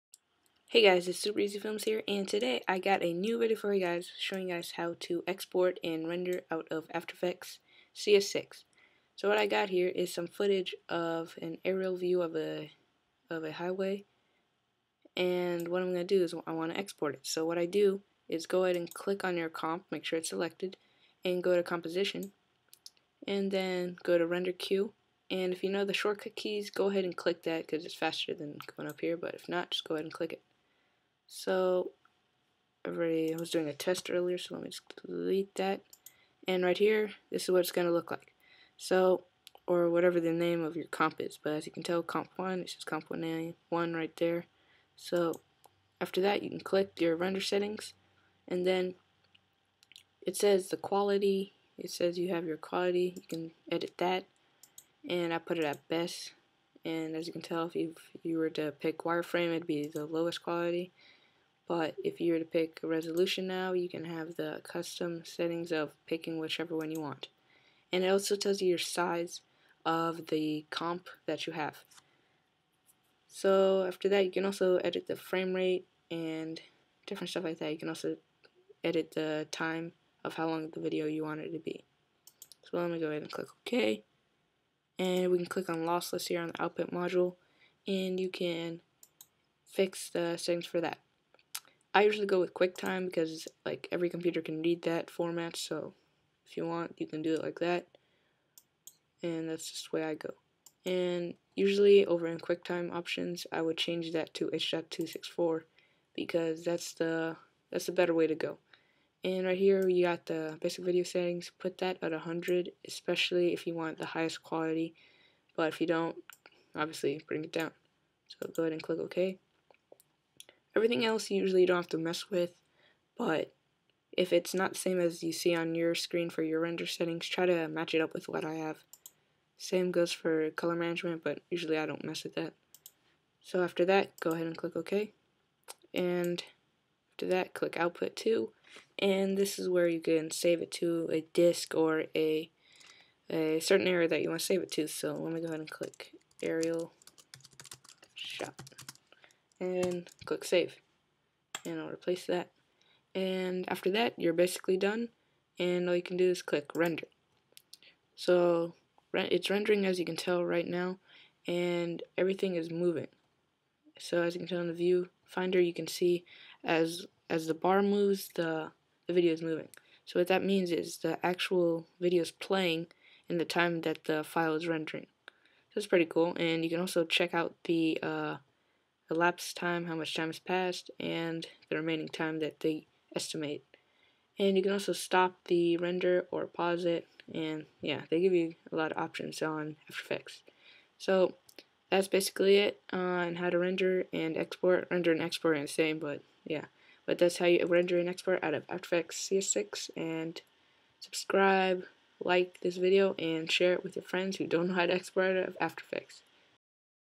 Render/Export From After Effects in Full HD
In this tutorial I show you guys how to export out of After Effects.